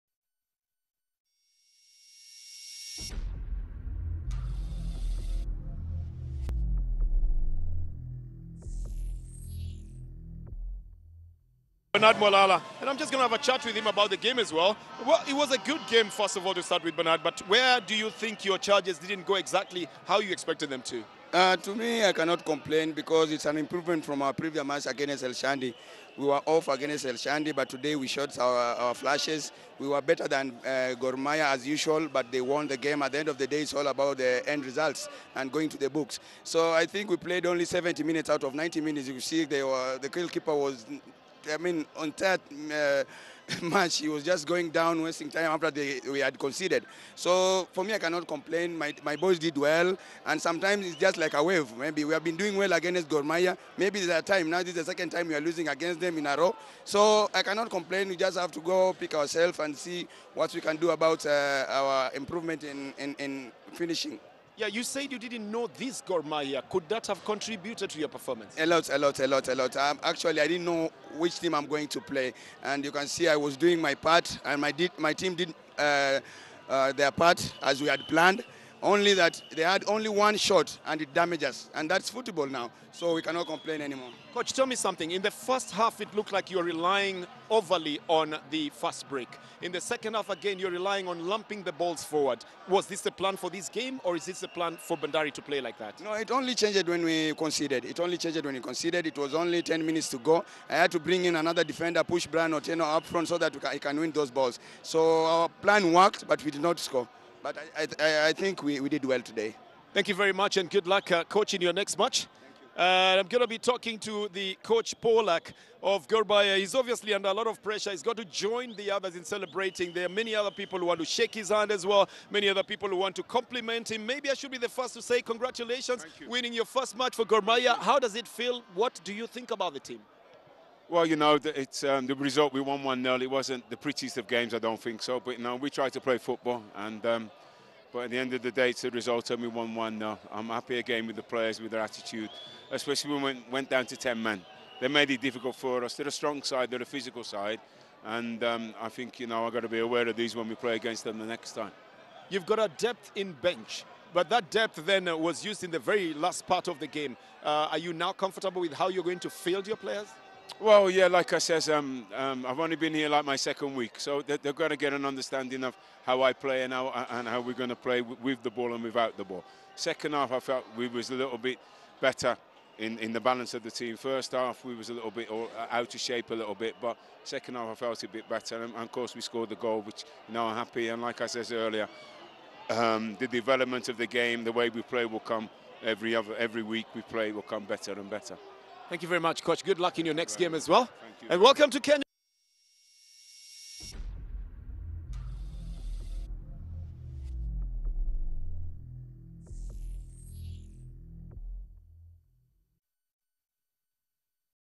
Gor Mahia FC 1-0 Bandari FC | COACHES' INTERVIEW | 2019 KPL Super Cup; Sun, 18.08.19
Watch the coaches' interviews from the 2019 KPL Super Cup match between Gor Mahia FC and Bandari FC which was played at the Kenyatta Stadium, Machakos, on Sunday, August 18, 2019.